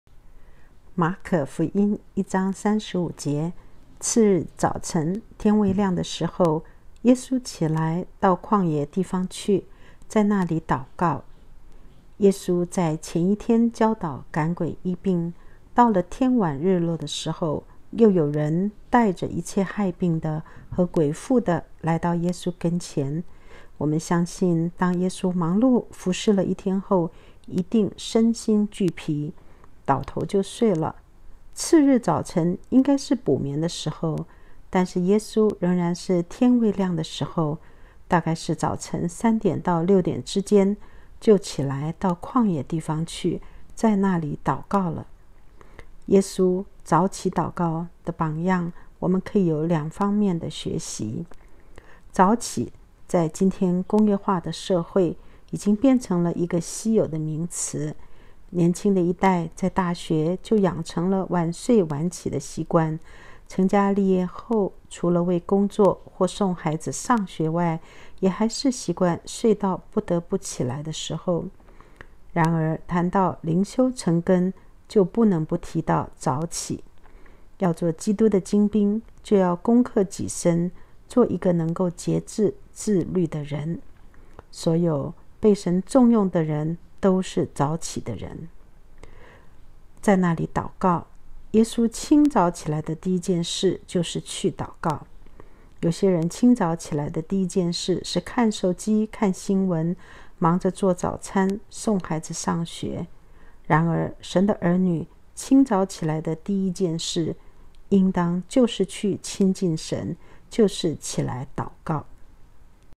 馬可福音5
馬可福音1:35‬
‪次日早晨、天未亮的時候、耶穌起來、到曠野地方去、在那裡禱告。‬

耶穌在前一天教導﹑趕鬼﹑醫病，到了天晚日落的時候，又有人帶著（原文時態顯示病人是繼續不斷的被帶到耶穌跟前來）一切害病的，和被鬼附的，來到耶穌跟前。我們相信當耶穌忙碌服事了一天後，一定身心俱疲﹑倒頭就睡了！「次日早晨」，應該是補眠的時候，但是，耶穌仍然是「天未亮的時候」，大約是早晨三點至六點之間，就起來到曠野地方去，在那裡禱告了！

接下來從耶穌「早起禱告」的榜樣，我們可以有兩方面的學習：
●「次日早晨，天未亮的時候，耶穌起來」：「早起」，在今天工業化的社會，已經變成了一個稀有名詞。年輕的一代，在大學就養成了晚睡晚起的習慣。成家立業後，除了為工作或送孩子上學外，也還是習慣睡到不得不起來的時候。然而，談到「靈修﹑晨更」，不能不提到要「早起」。要作基督的精兵，就要攻克己身，作一個能夠節制、自律的人。聖經上，凡是被神用的人，都是「早起」的，亞伯拉罕﹑摩西﹑大衛王﹑但以理﹑彼得 ﹑保羅﹑約翰…。此外，歷世歷代的聖徒，凡是愛主﹑被主使用的，也都是「早起」的，例如，約翰衛斯理﹑戴德生﹑施達德﹑倪柝聲﹑宋尚節﹑林道亮牧師…。

●「到曠野地方去，在那裡禱告。」：耶穌清早起來的第一件事是去「禱告」！有些人清早起來的第一件事是看手機﹑看新聞﹑忙做早餐﹑送孩子上學…，然而，神的兒女清早起來的第一件事，應當是去「親近神」！ See Less

Comments